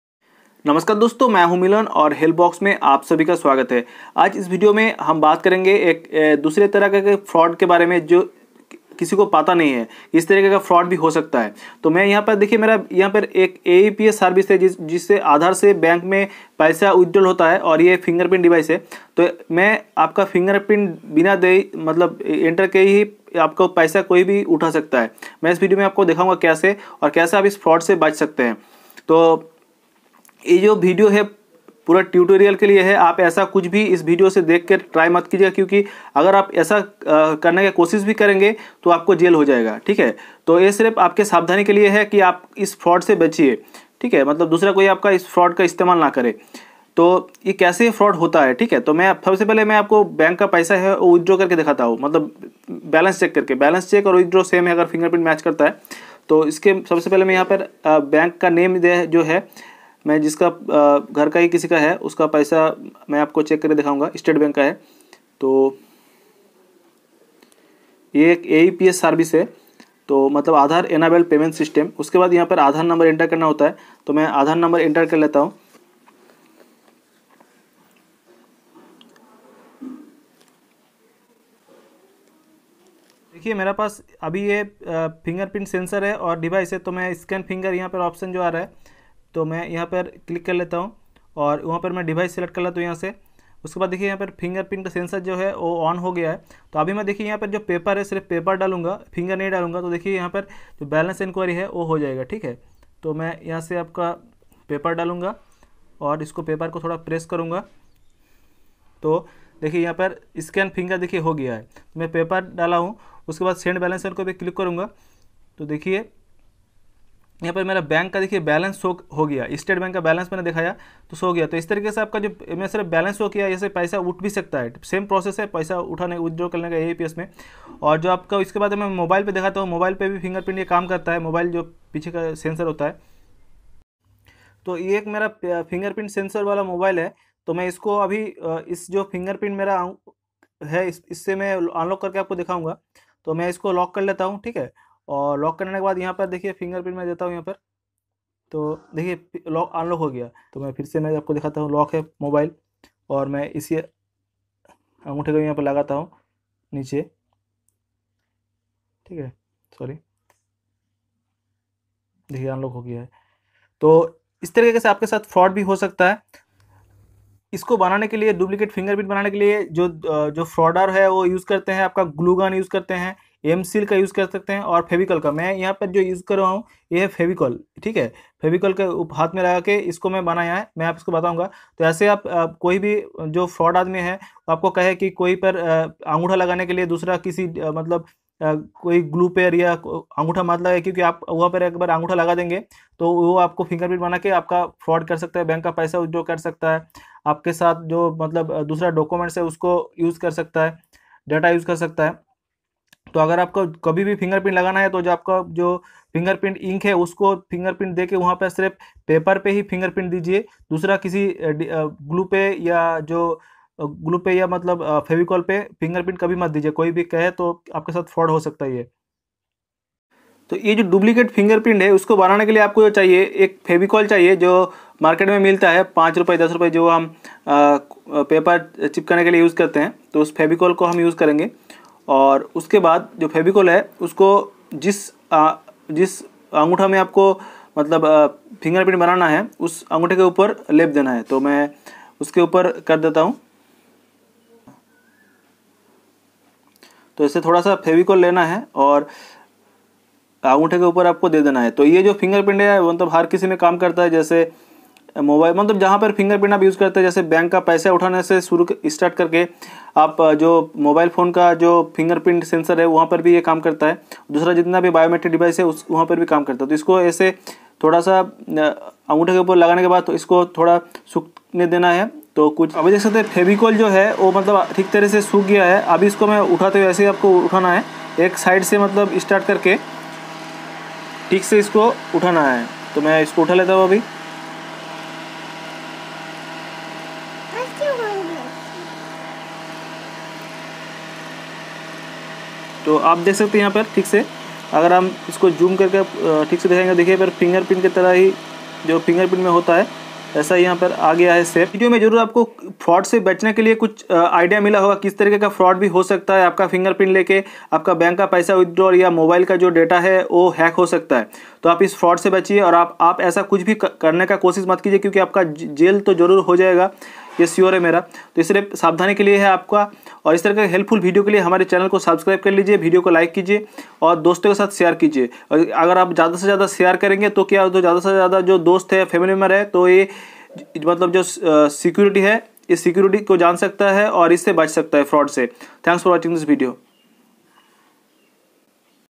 साबधान Finger print Fraud / आपका नकल अंगूठा छाप से बैंक खाली हो सकते है
Be alert never put your finger on any unknown material like glue, gum and jelly type element.

Because cheater can take your finger print and it can use for illegal purposes like bank fraud, mobile security and on other biometric device.